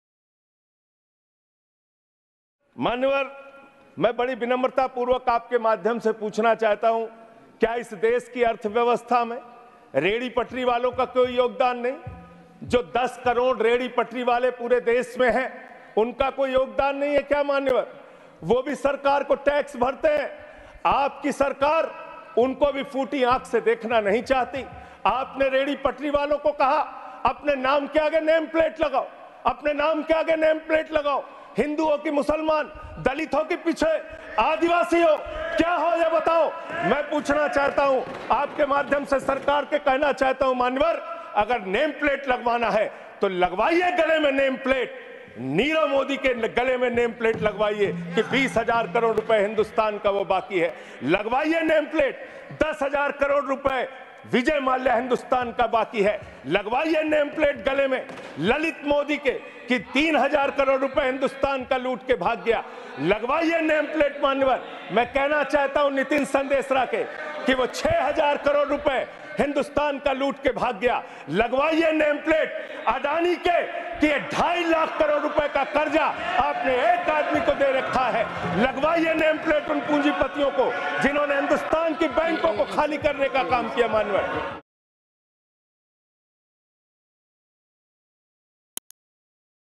If You Want To Keep Name Plates AAP's Sanjay Singh Attack BJP Government
"If You Want To Keep Name Plates...": AAP's SanjaySingh Attack